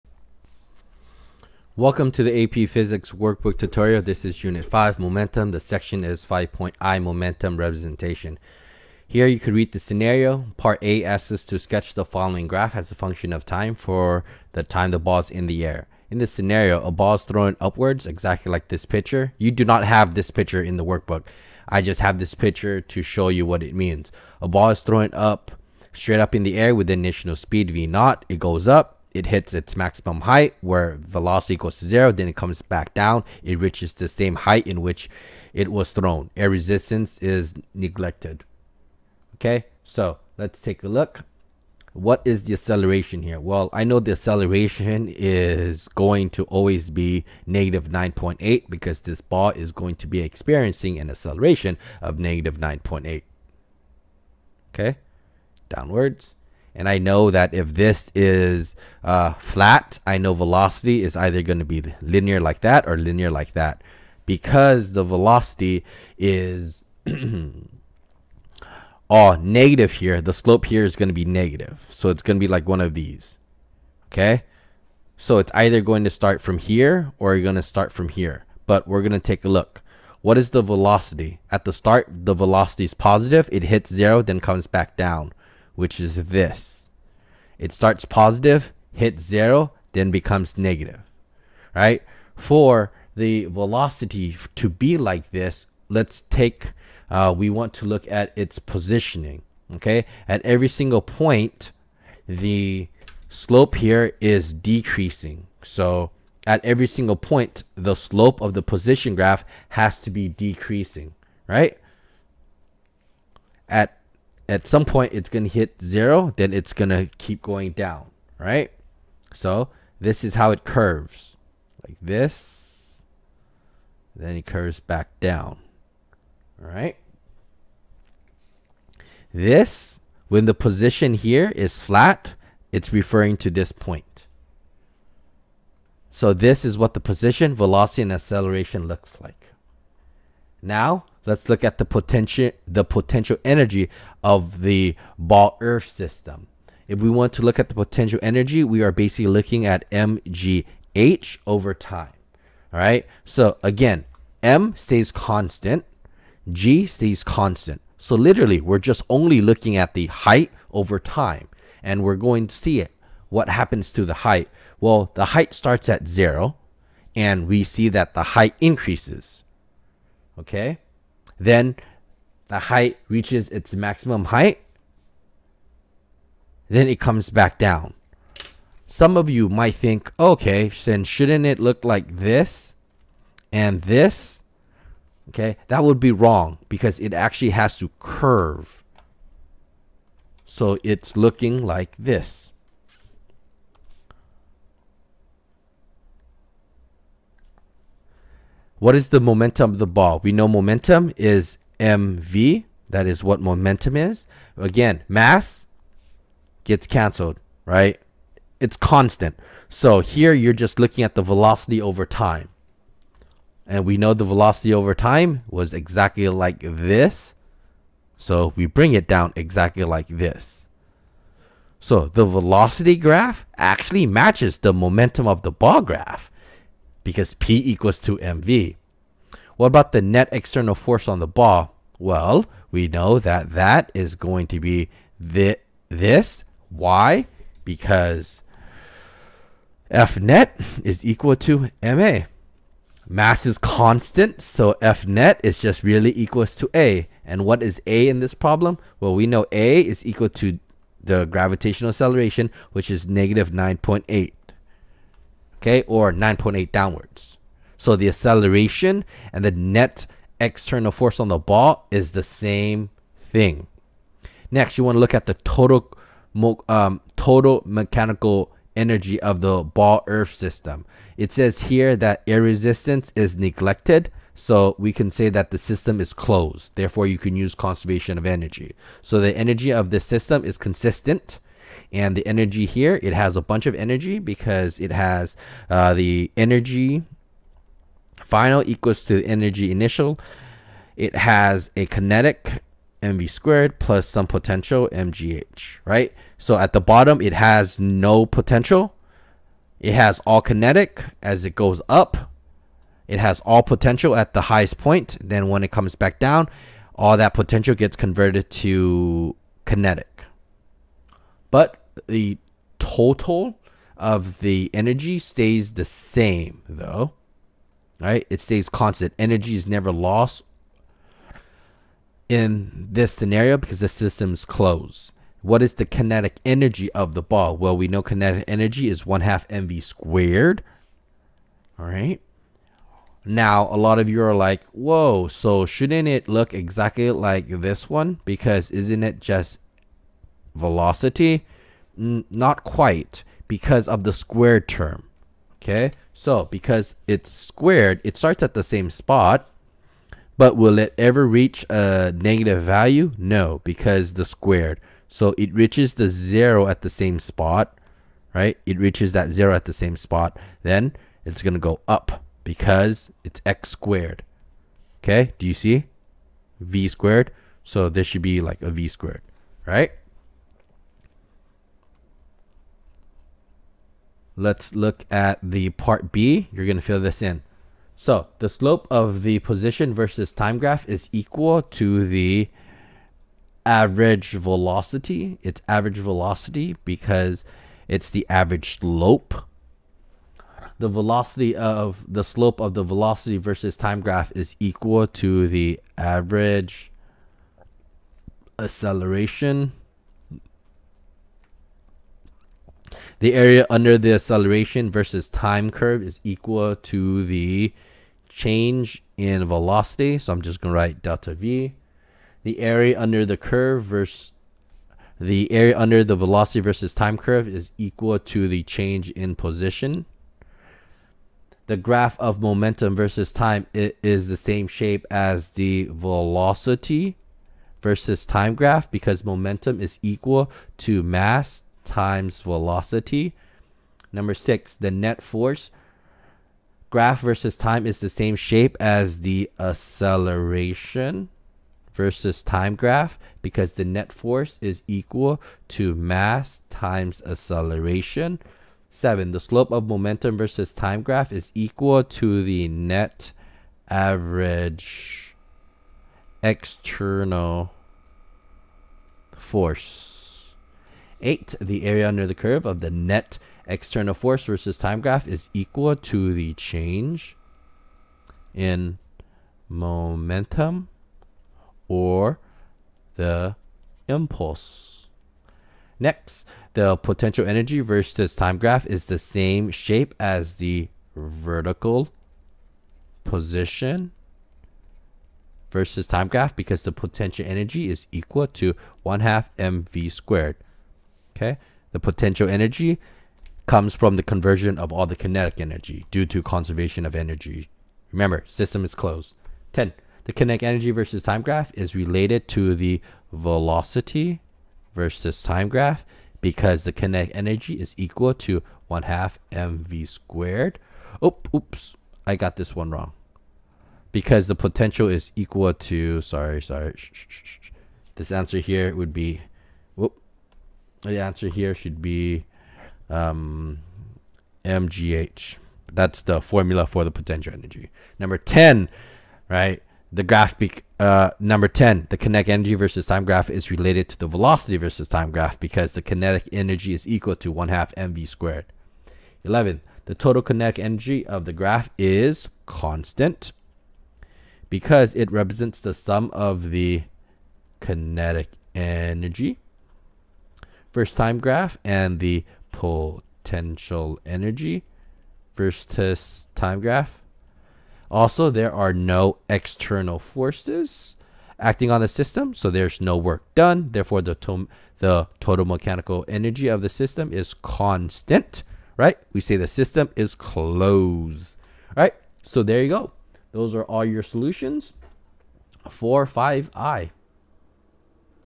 AP Physics Workbook 5.I Momentum Representations
This is the video that cover the section 5.I in the AP Physics 1 Workbook.
Topic over:
1. position, velocity, acceleration graph
2. Potential energy and kinetic Energy
3. Momentum
4. Net external Force
5. Total Mechanical Energy of the System

This is a packet given to student by the CollegeBoard and for student to do. Student are to give the packet an honesty attempt and effort. This video is created to supplement the learning and to provide assistance to student in the learning process and is not suppose to be use as a cheating tool.